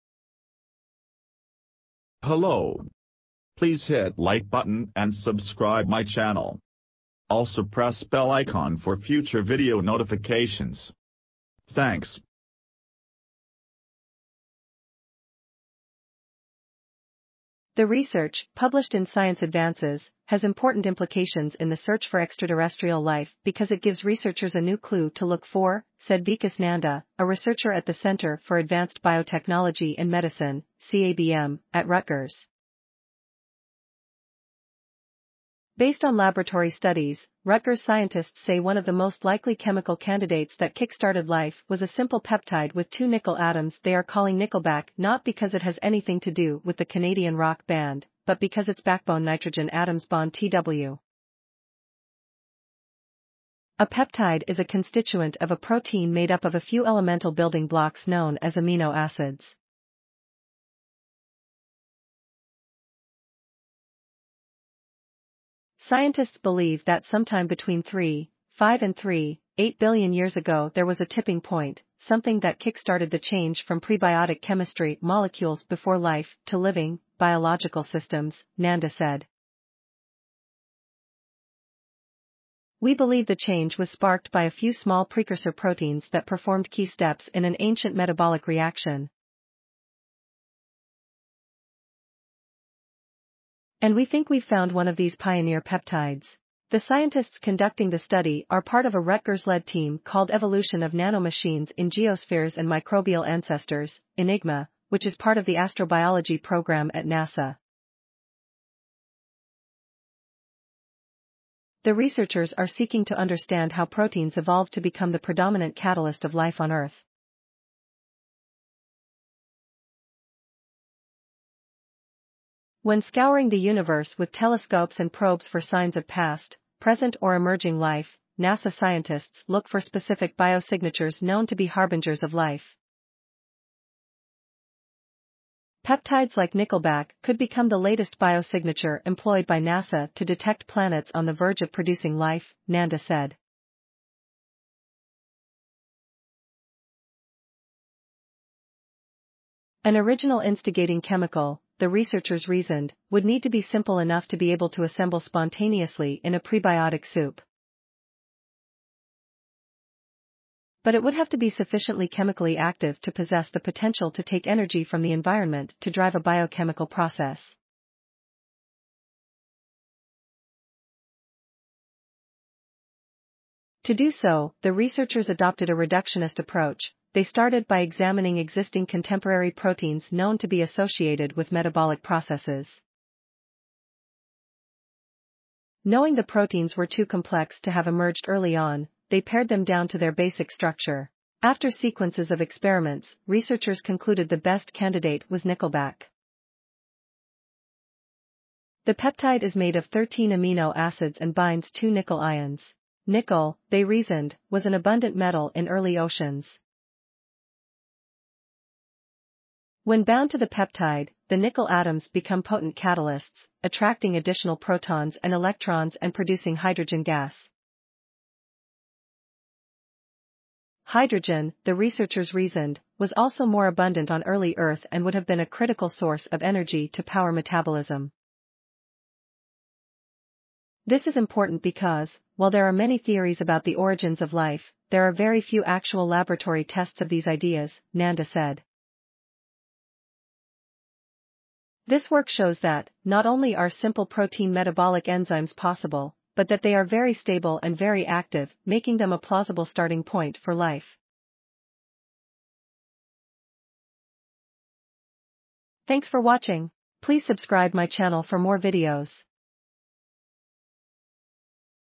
Rutgers scientists identify substance that may have sparked life on Earth
The research, published in Science Advances, has important implications in the search for extraterrestrial life because it gives researchers a new clue to look for, said Vikas Nanda, a researcher at the Center for Advanced Biotechnology and Medicine (CABM) at Rutgers. Based on laboratory studies, Rutgers scientists say one of the most likely chemical candidates that kickstarted life was a simple peptide with two nickel atoms they are calling “Nickelback” not because it has anything to do with the Canadian rock band, but because its backbone nitrogen atoms bond two critical nickel atoms. A peptide is a constituent of a...